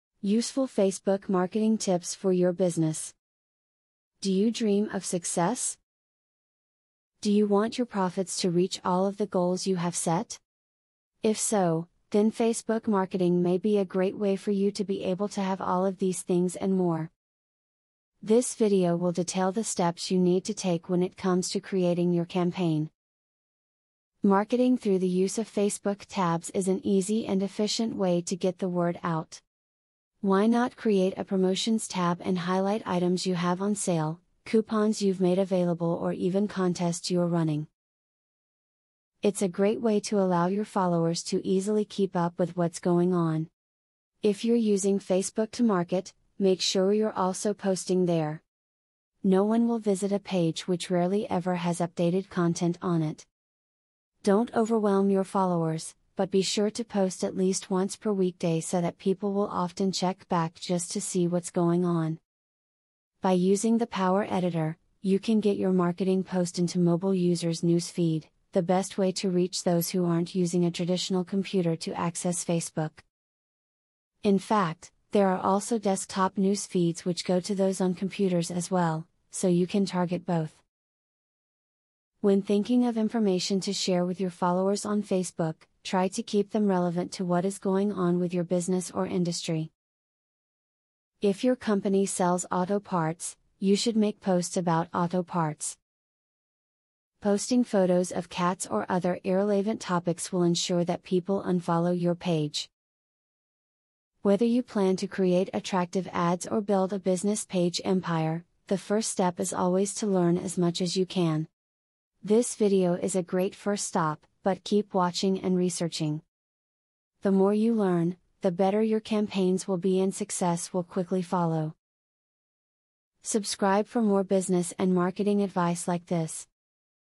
Useful Facebook Marketing Tips For Your Business.
Useful Facebook Marketing Tips For Your Business. Do you dream of success? Do you want your profits to reach all of the goals you have set? If so, then Facebook marketing may be a great way for you to be able to have all of these things and more. This video will detail the steps you need to take when it comes to creating your campaign.